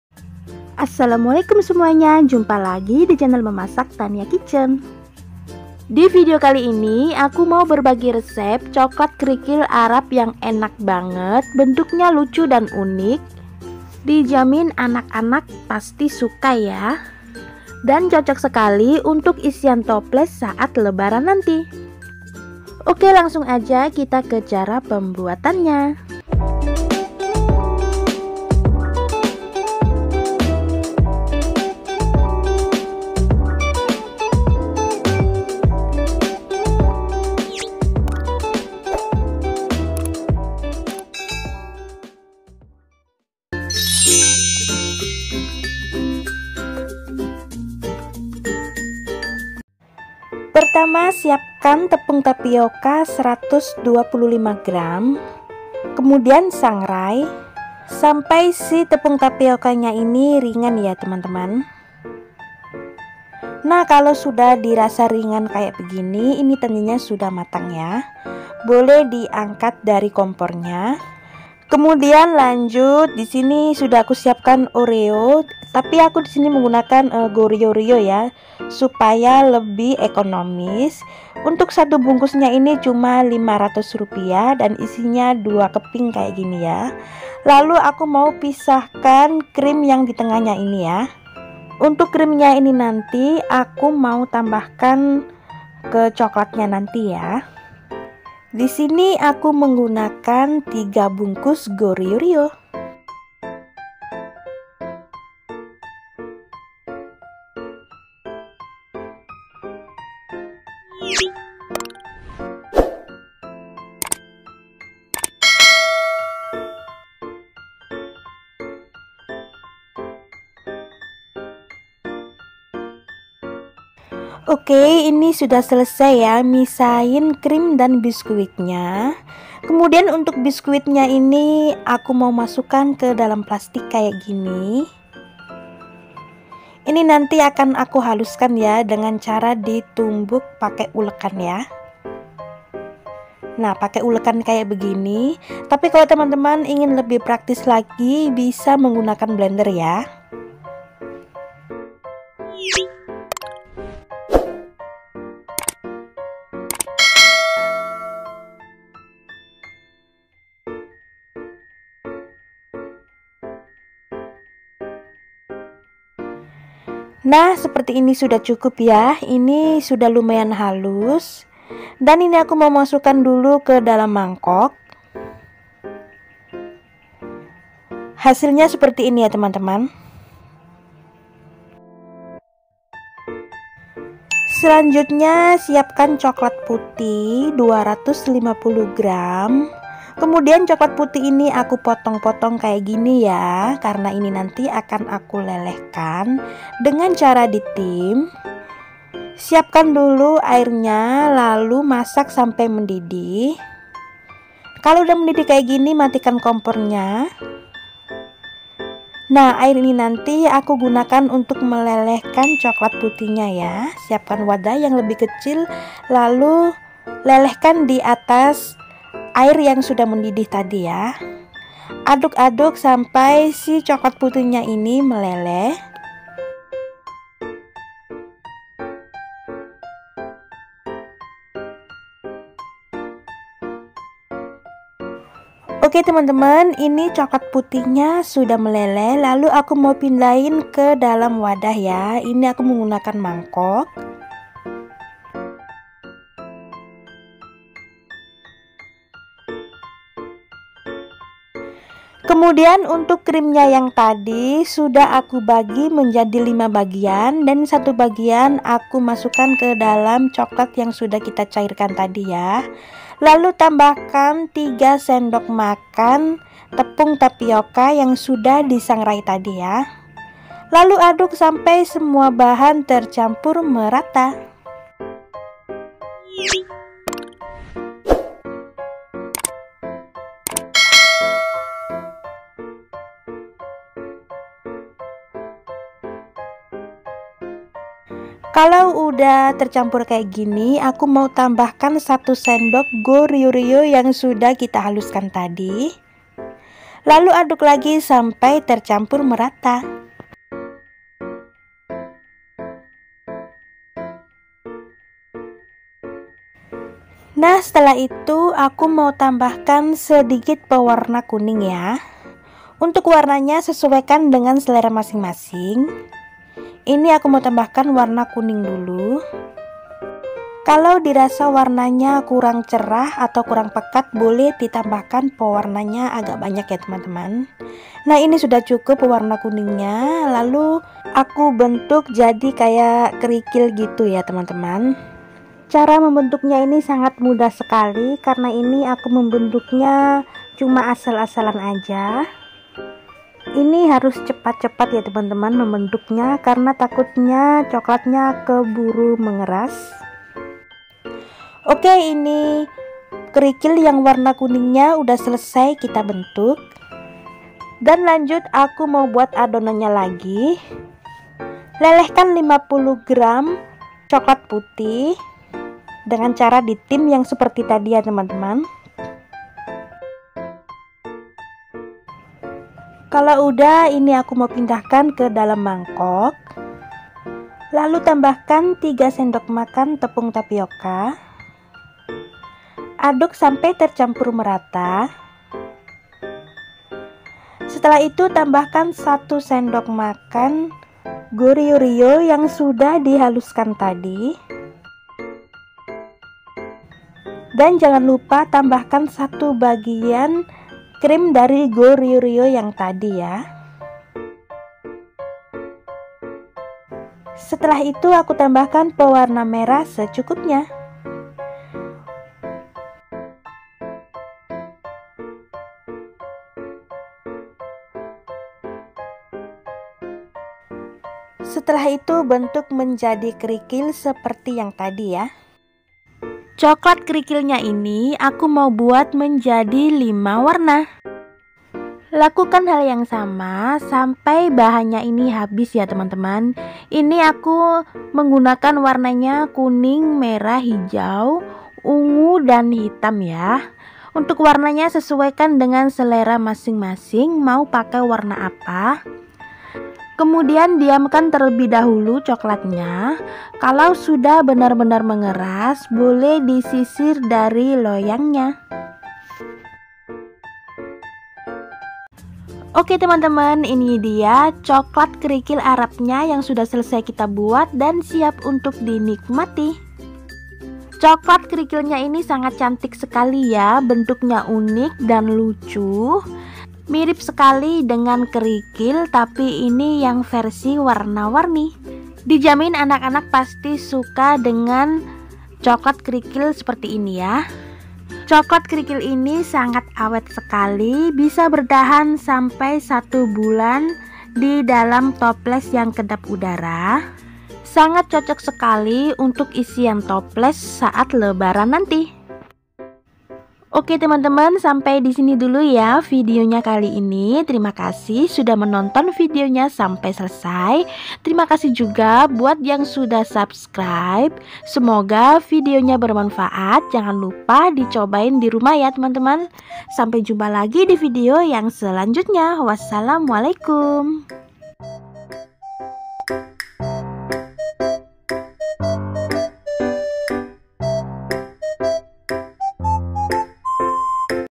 RESEP COKLAT KERIKIL KHAS ARAB || IDE ISIAN TOPLES LEBARAN
bu ibu pasti udah pusing nih mikirin kue untk isian toples lebaran nanti. coba buat ini ajah bun, coklat kerikil khas Arab yg enak bgt. bentuknya unik mirip kerikil tp warna warni cantik bgt. anak2 pasti sukak, berikut bahan yg d perlukan :
1.coklat putih 250 gr
2.tepung tapioka 125 gr
3.oreo/goriorio
4.pewarna makanan secukupnya

selamat mencoba
happy cooking💕